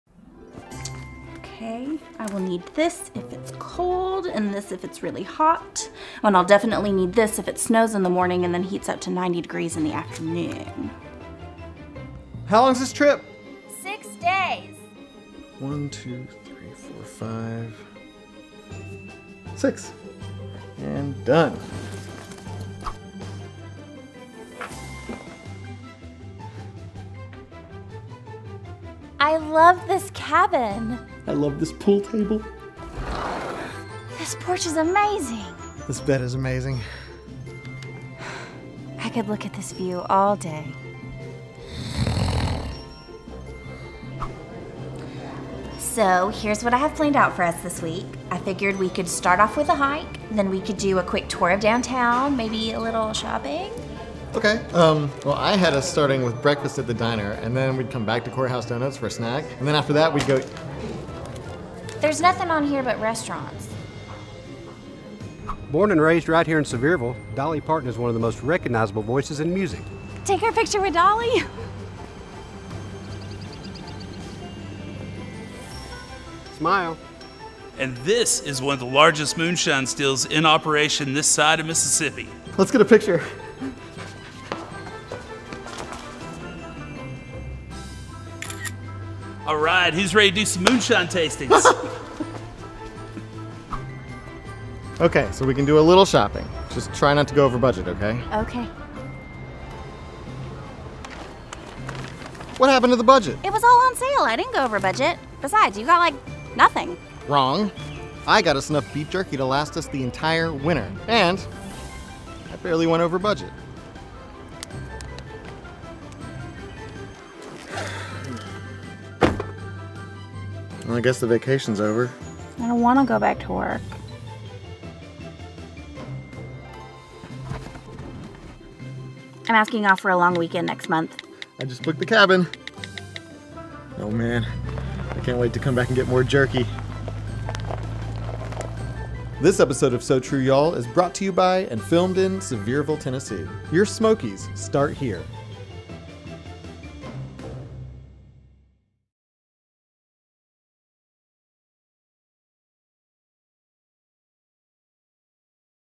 When Couples go on Vacation
One is a planner. The other is too, but mostly for food and naps. 😂